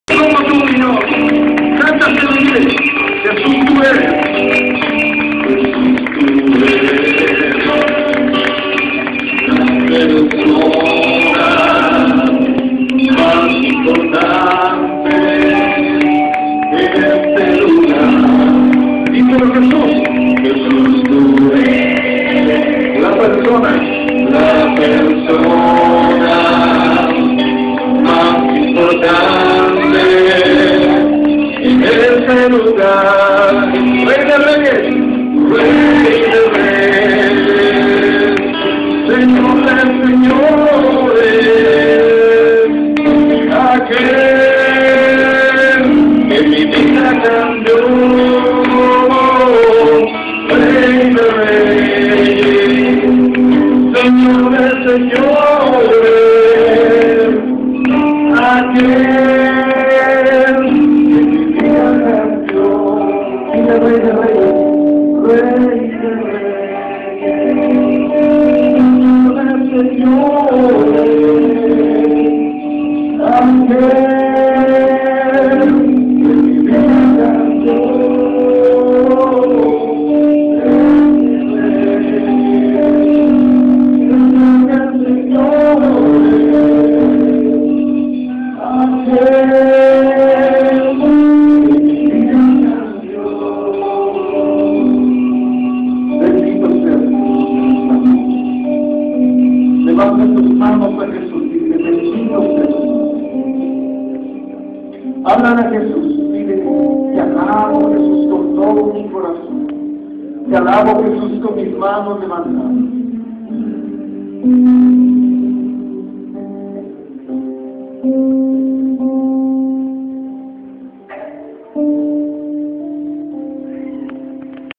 Hora Santa Regional Jufra 2013
Ésta es la entrada de Nuestro Señor Jesucristo en el Encuentro Regional Jufra llevado a cabo en San Martín Texmelucan, el día sábado 23 de noviembre de 2013 con la participación del Ministerio de canto y música Transfiguración.